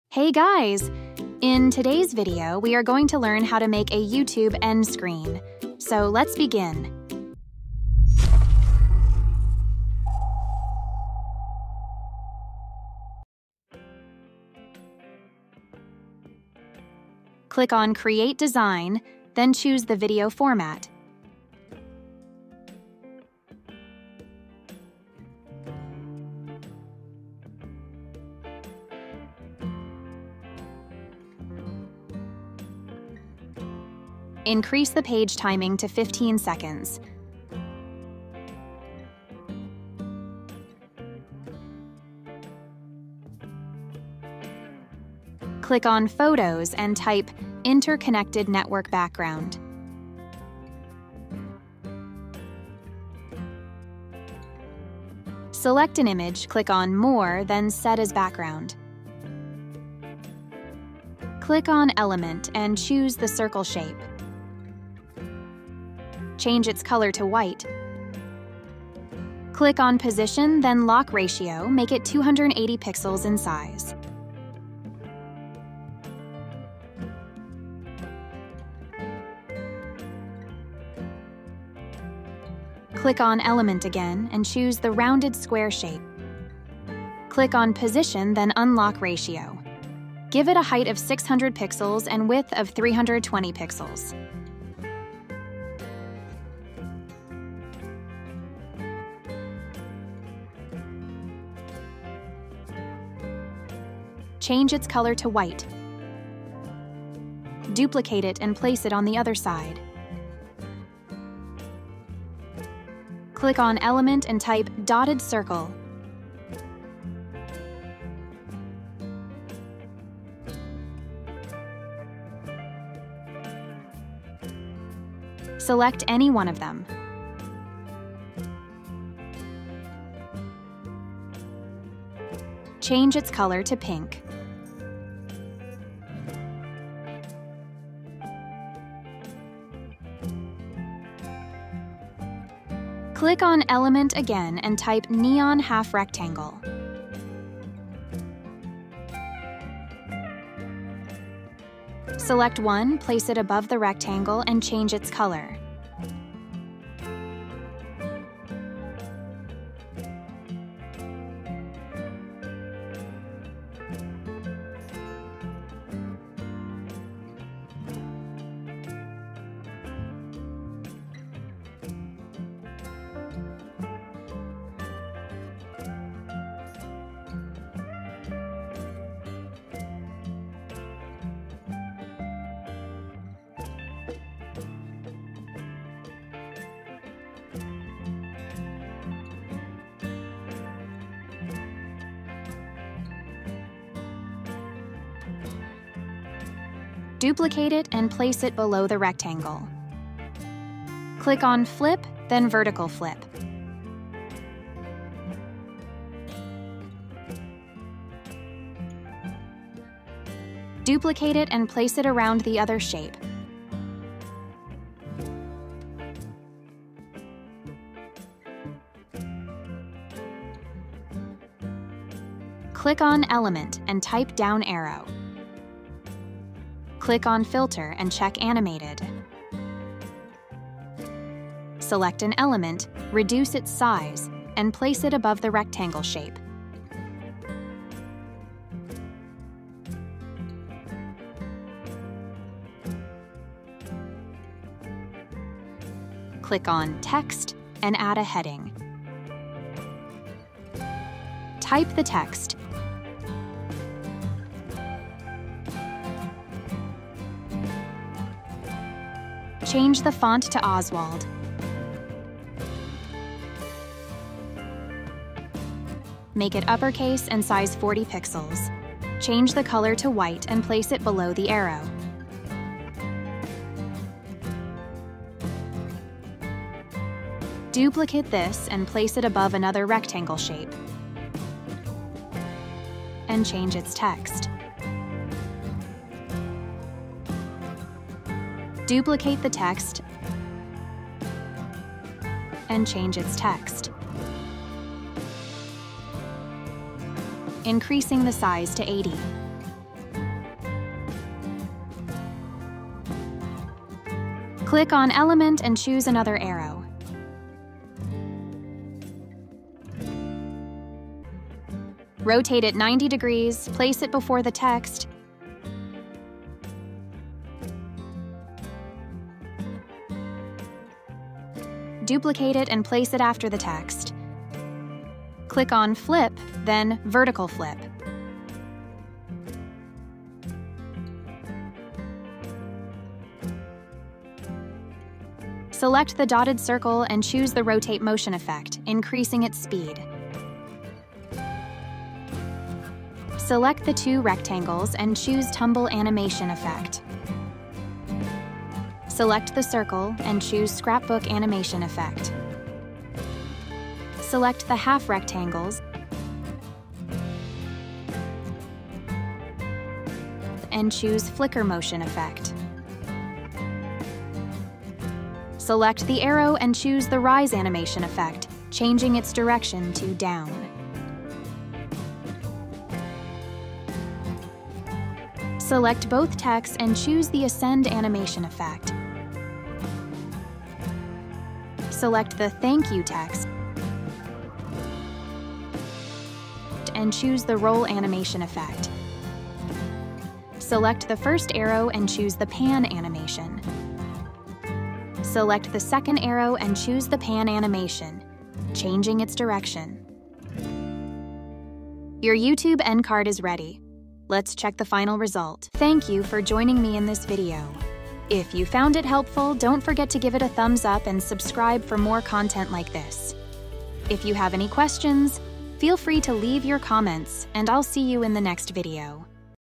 How to make YouTube End Card on Canva
Learn how to create a  YouTube End Screen using Canva in this easy-to-follow tutorial! Whether you're looking to enhance your YouTube videos with a professional outro or you're new to creating end screens, this tutorial has you covered. Discover step-by-step instructions on how to make an outro for YouTube videos, design a YouTube end screen, and customize it to fit your channel's branding. With Canva's user-friendly interface and our straightforward guide, you'll be creating stunning end screens in no time. Elevate your YouTube content with an engaging outro that keeps viewers coming back for more!